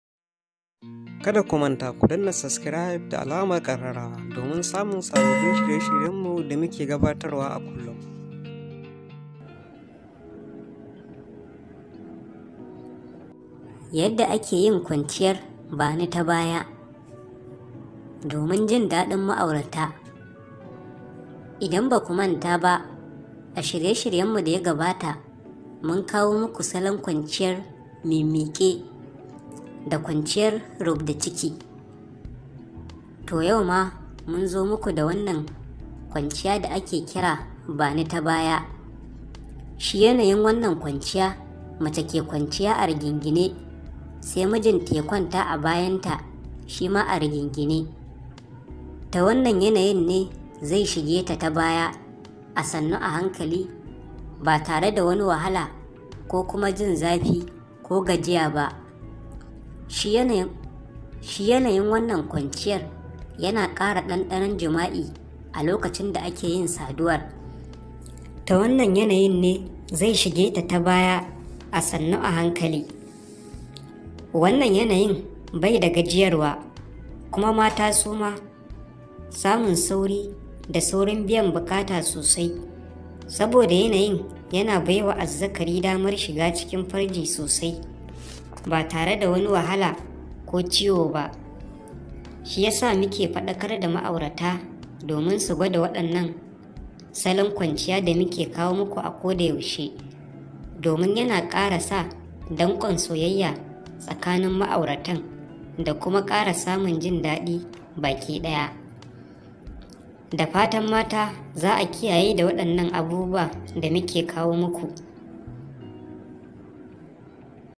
Wannan video anyishi ne domin ma'aurata yadda ake yin kwanciyar bani bura ta baya
Mungode da ziyartar channel dinmu, ilimantarwa tv

kar ku manta kuyi sharing zuwa abokaina da 'yan uwa ta WhatsApp da kuma Facebook da sauran shafukan social media.